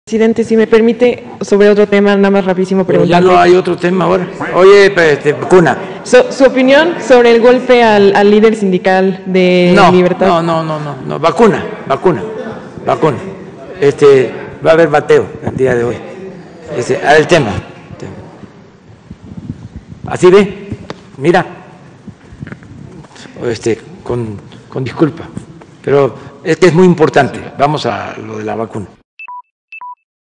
Hoy sólo sobre la vacuna 🦆
Hoy sólo quiso responder preguntas sobre la vacuna, pero ya también habló de la recuperación económica en “v”, que ya no hay desempleo, de las benditas remesas y de los programas sociales 🦆